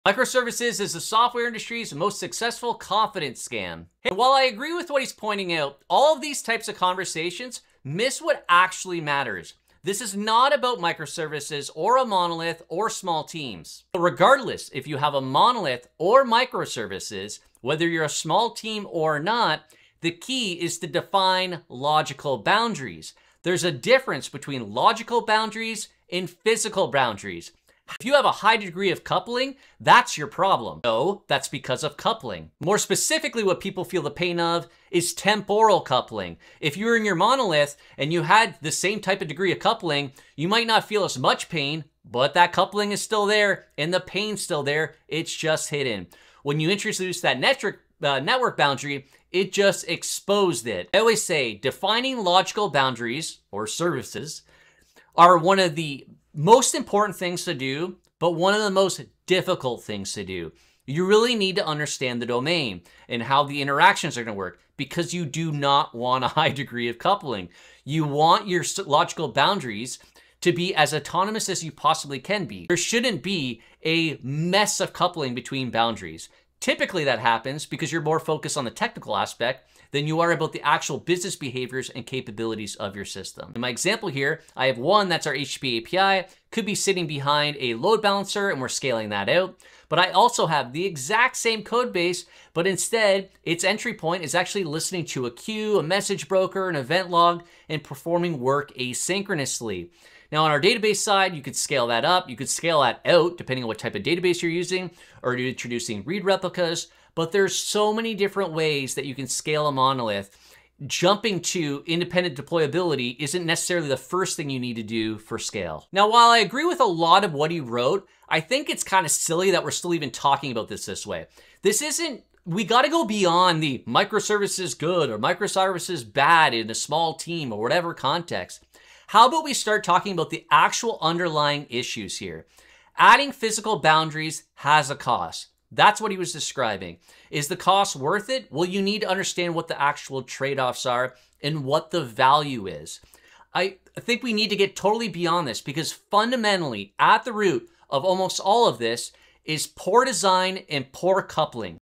Why “Microservices” Debates Miss the Point
Microservices vs monolith is the wrong debate. In this video, I react to DHH’s take and break down the distinction most people miss: logical boundaries vs physical (deployment) boundaries. We’ll talk about why “synchronizing a fleet” happens, how coupling (especially temporal coupling) creates the real pain.

0:00 Intro
3:04 Operational Cost
4:09 Coupling
7:0 Scaling